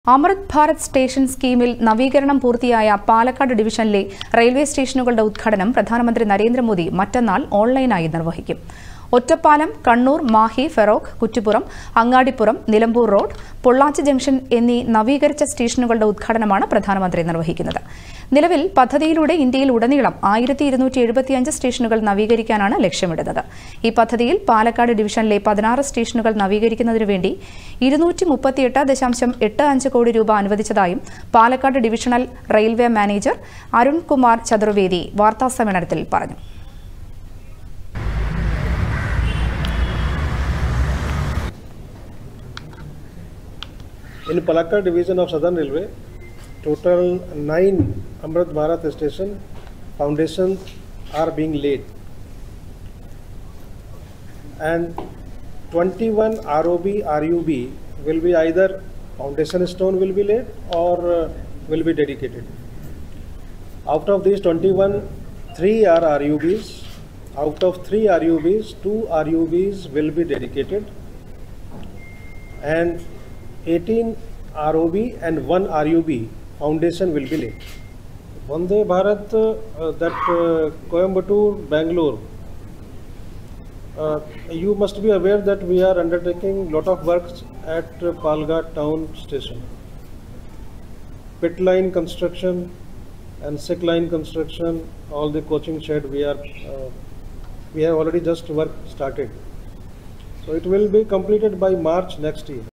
അമൃത് ഭാരത് സ്റ്റേഷൻ: പാലക്കാട് ഡിവിഷനിലെ റെയിൽവേ സ്റ്റേഷനുകളുടെ ഉദ്ഘാടനം പ്രധാനമന്ത്രി നിർവഹിക്കും
അമൃത് ഭാരത് സ്റ്റേഷൻ സ്കീമിൽ നവീകരണം പൂർത്തിയായ പാലക്കാട് ഡിവിഷനിലെ റെയിൽവേ സ്റ്റേഷനുകളുടെ ഉദ്ഘാടനം പ്രധാനമന്ത്രി നരേന്ദ്രമോദി മറ്റെന്നാൾ ഓൺലൈനായി നിർവഹിക്കും.